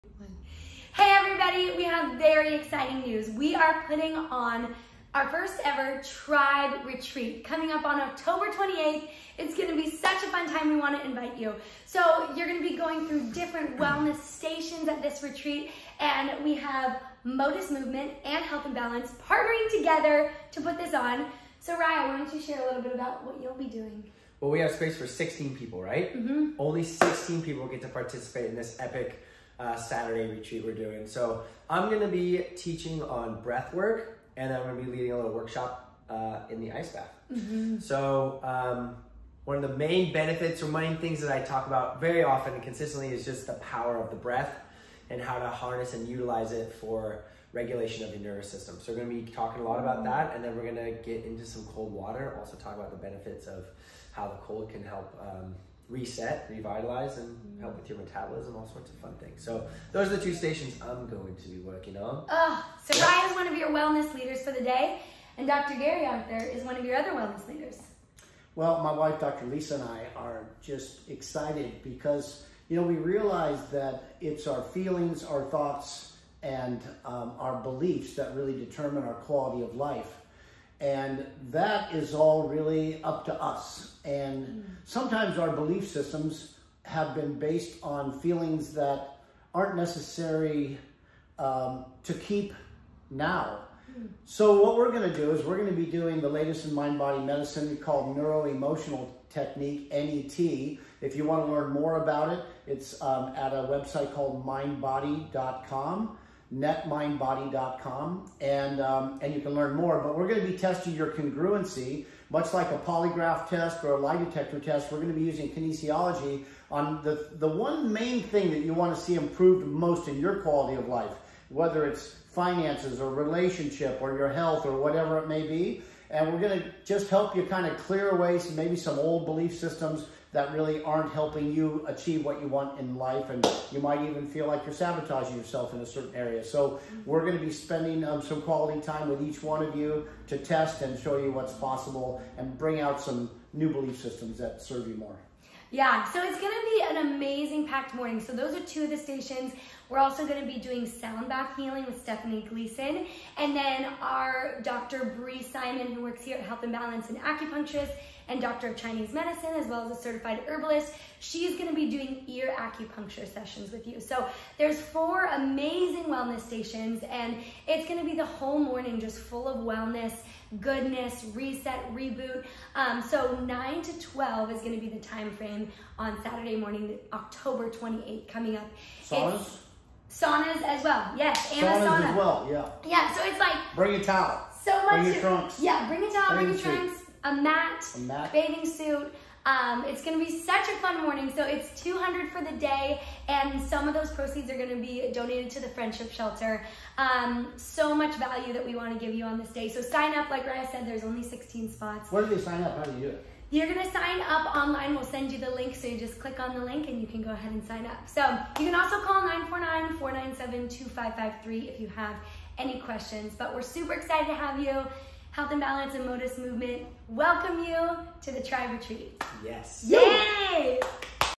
We invite you to the Tribe Retreat!
Inviting you to join us at the Tribe Retreat on October 28th for a day of wellness, refreshment, rejuvenation and whole body care!